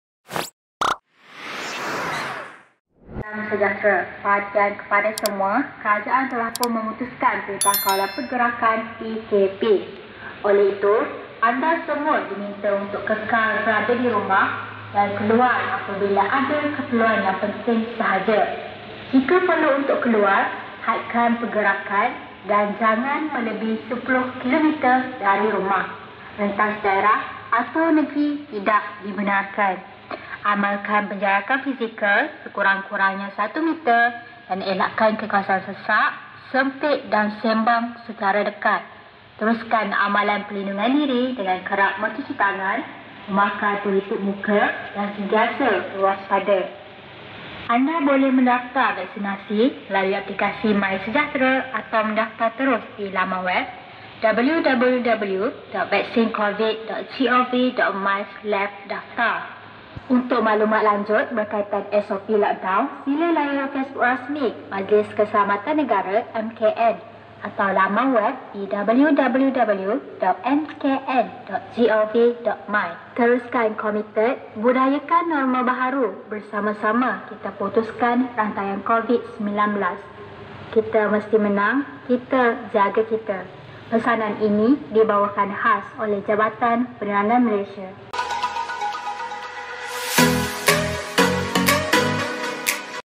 DAERAH KUALA NERUS | INFO ON WHEELS ( IOW) PKP 3.0 2021 [17.6.2021]
INFO ON WHEELS ( IOW) PKP 3.0 2021/ VAKSIN /PROKLAMASI  DARURAT PEJABAT PENERANGAN DAERAH KUALA NERUS

Parlimen : P.035 Kuala Nerus
DUN : N.12 Bukit Tunggal
Tarikh : 17 Jun 2021
Masa : 8.30 pg - 10.30 pg

Lokasi :
1. Mydin Mall
2. Dataran Austin
3. Dataran Rimba
4. Taman Desa Jaya
5. Pasaraya PXR

Jumlah lokasi : 5
Jumlah hebahan :10 kali
Jumlah audiens : 250 orang